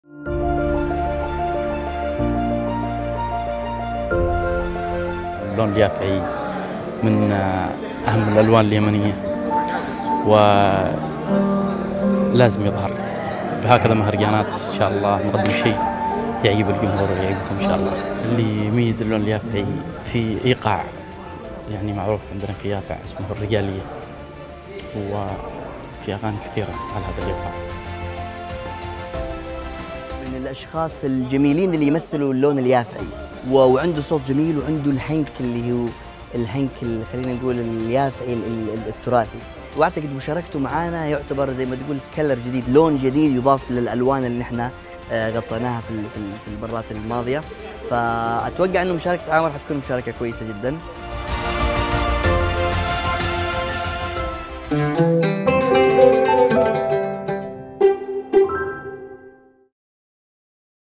في تدريبات اليوم، يقدم الفنان عامر اليافعي الأغنية المتجددة 🎶
اليوم، تدرب فريقنا على أغنية عريقة عمرها أكثر من 200 عام، بينما لا تزال تحتفظ بروحها الجميلة وبريقها المميز ✨.

في تدريبات اليوم، يقدم الفنان عامر اليافعي الأغنية المتجددة 🎶، مضيفًا إليها المزيد من البهجة والرقة والإتقان، ناقلًا إياها إلى آفاق رحبة جديدة من الإبداع، واضعًا بصمة جديدة على حفلات السيمفونيات التراثية 🎼.

لكن هل عرفتم ماهي هذه المقطوعة؟ شاركونا في التعليقات 🤩👇!